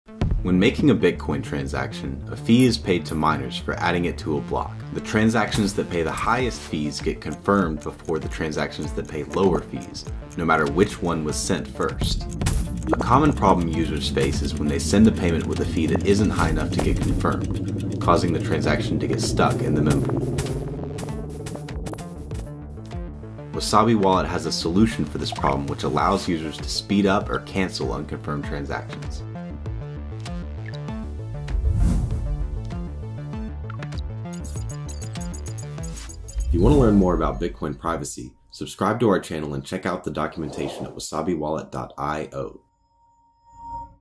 How to Speed up or Cancel Your Bitcoin Transactions
You won't have to count the hours passing by waiting for your transaction that's stuck in the mempool. Wasabi has an easy solution.